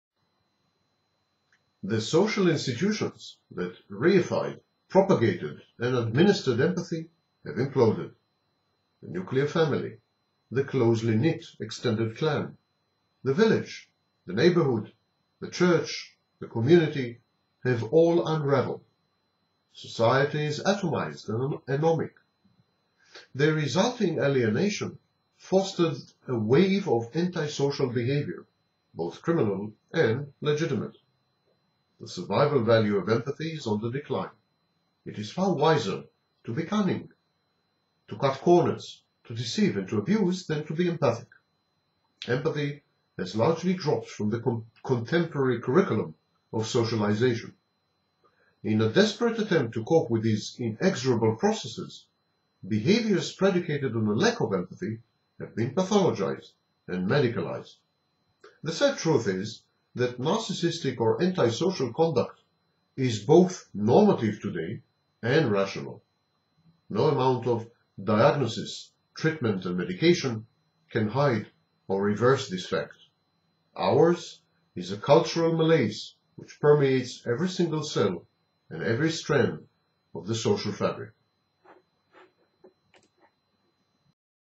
The Implosion of Empathy
The social institutions that reified, propagated and administered empathy have imploded. The nuclear family, the closely-knit extended clan, the village, the neighborhood, the Church- have all unraveled. Society is atomized and anomic. The resulting alienation fostered a wave of antisocial behavior, both criminal and "legitimate". The survival value of empathy is on the decline. It is far wiser to be cunning, to cut corners, to deceive, and to abuse - than to be empathic. Empathy has largely dropped from the contemporary curriculum of socialization. In a desperate attempt to cope with these inexorable processes, behaviors predicated on a lack of empathy have been pathologized and "medicalized". The sad truth is that narcissistic or antisocial conduct is both normative and rational. No amount of "diagnosis", "treatment", and medication can hide or reverse this fact. Ours is a cultural malaise which permeates every single cell and strand of the social fabric.